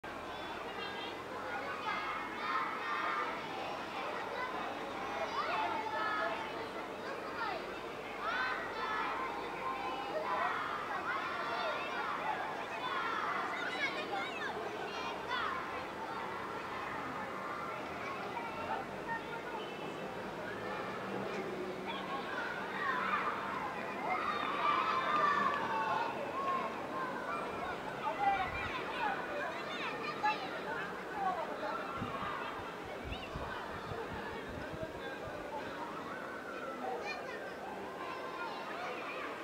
Femijet duke luajtur
Femijet duke luajtur ne oborrin e shkolles Pjeter Arbnori Fushe-Arrez.